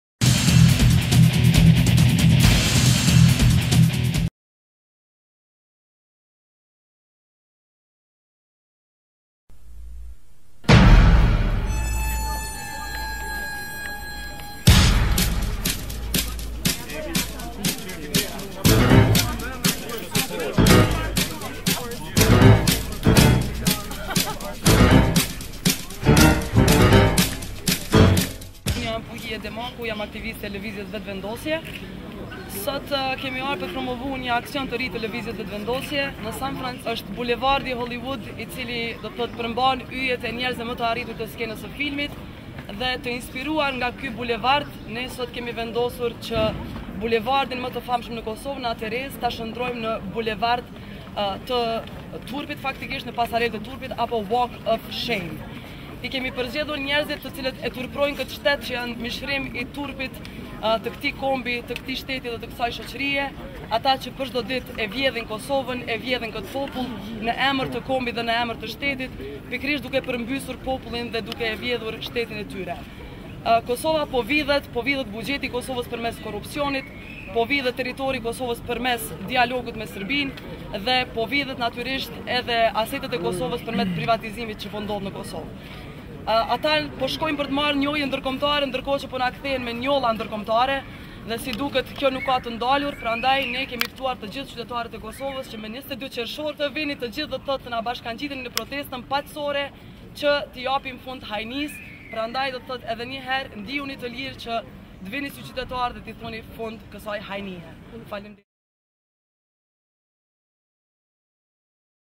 Lëvizja VETËVENDOSJE! Aksion "Pasarela e Turpit" 17 Qershor 2011
Sot aktivistët e Lëvizjes VETËVENDOSJE! në sheshin Nëna Terezë, realizuan aksionin simbolik "Pasarela e Turpit" me fotografitë e Yjeve të hajnisë së pronës publike,  të mirëqenies së qytetarëve dhe të së ardhmes së Kosovës. Midis tyre më të dalluarit në famën e tyre janë: Hashim Thaçi, Besim Beqaj, Edita Tahiri, Bredri Hamza, Hajredin Kuçi, Bajram Rexhepi.